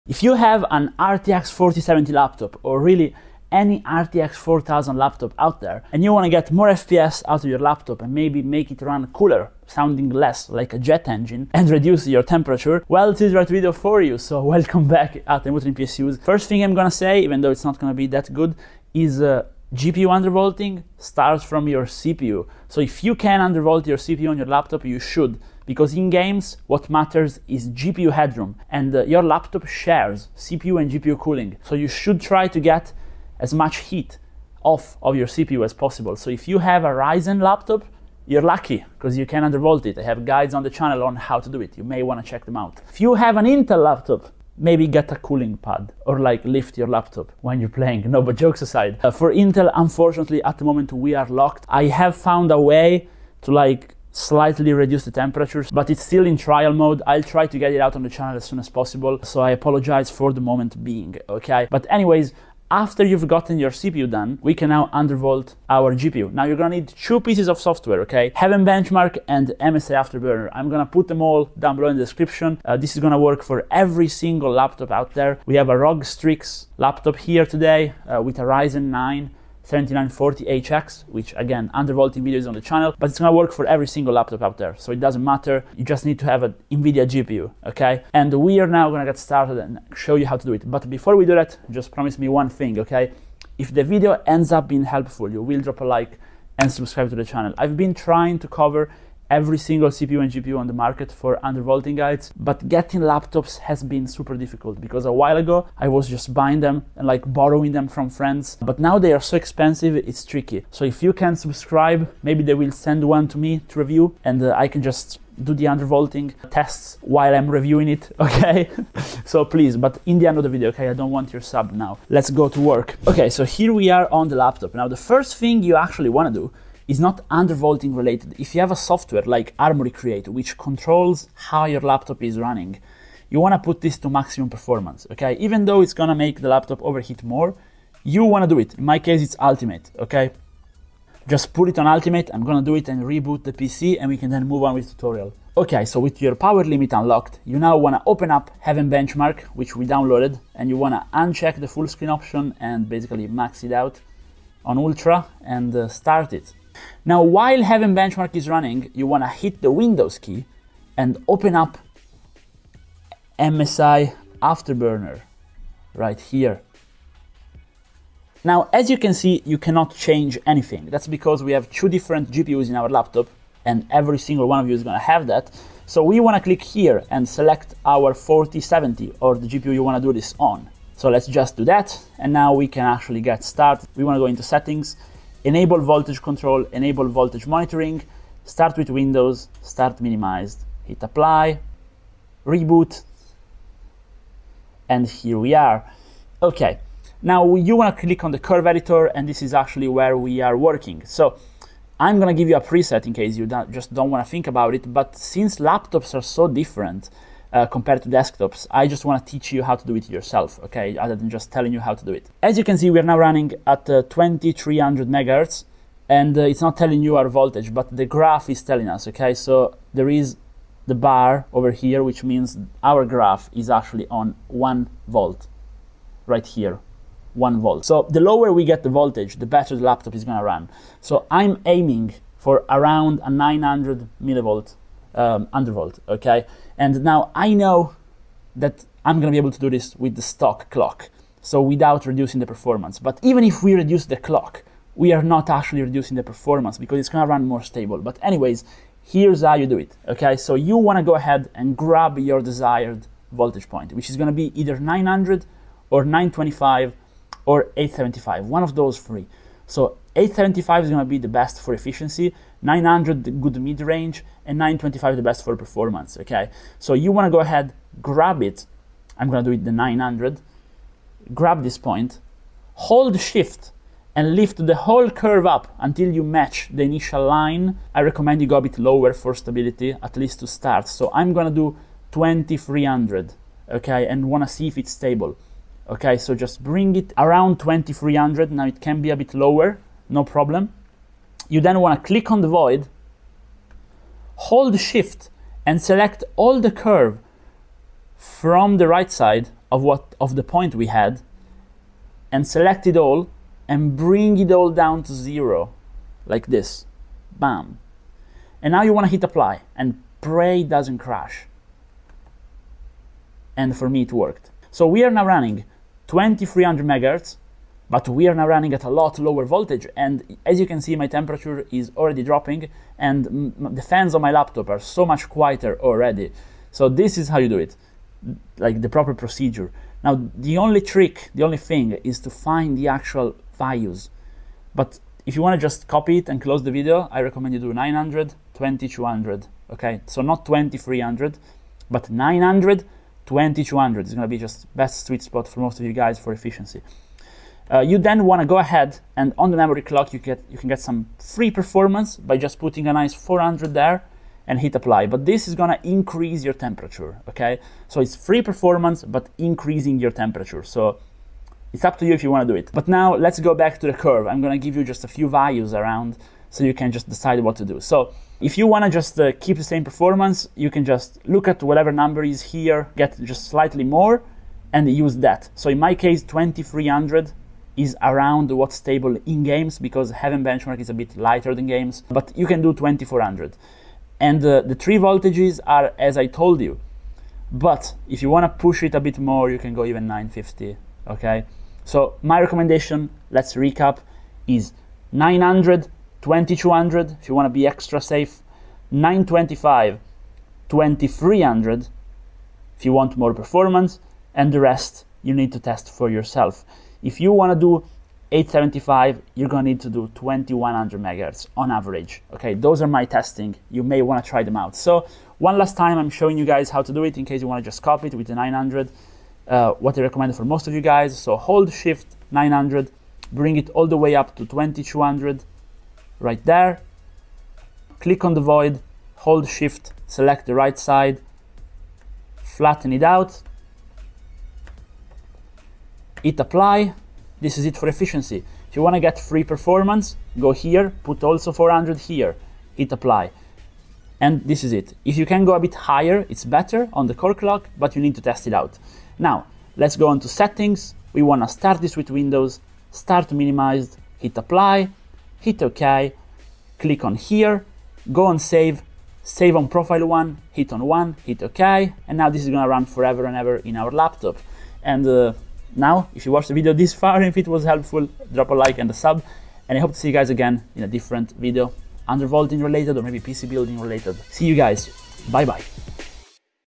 Undervolt your Laptop RTX 4070 for more FPS and Lower Temperature! - Tutorial
Undervolting really seems to be the best option in terms of GPU optimization for the RTX 4000 series. Even on laptops. Be it a 4090, 4080, 4070 Ti or even a 4060 Ti, if you do it properly, you’re gonna lose virtually 0 performance or even get an increase, while dramatically reducing heat and power consumption. Today we are featuring a RTX 4070 8GB in an Asus RoG Laptop.

This tutorial will actually work on EVERY RTX 4070 series laptop

The software in the background is Heaven Benchmark, here's the

This is my choice of thermal paste:

Here's the electric screwdriver I use for every PC I build:

This is the small compressor I recommend to clean PCs: